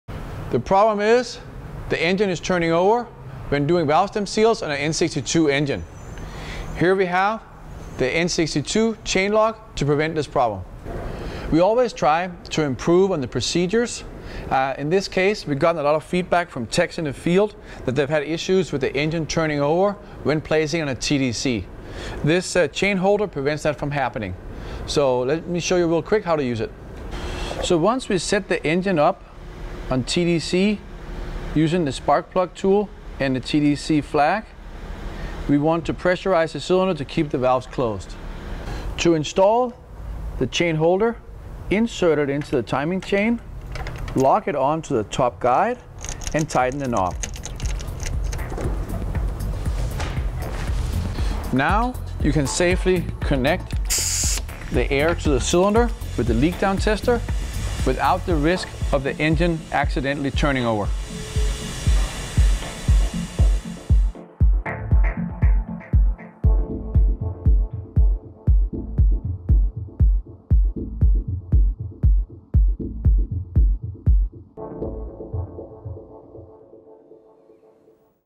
N62 Chain Lock Tool
AGA Tools has manufactured a N62 Chain Lock Tool to prevent the engine from turning over during a valve stem seal repair. By setting the engine to TCD and pressuring the cylinder, if the TDC is not perfectly aligned the engine can turn over in either direction. Our N62 Chain Lock Tool will prevent this from happening. Before the cylinder is pressurized, insert the chain lock tool to lock the chain and inhibit it from turning over.

Questions About This Product?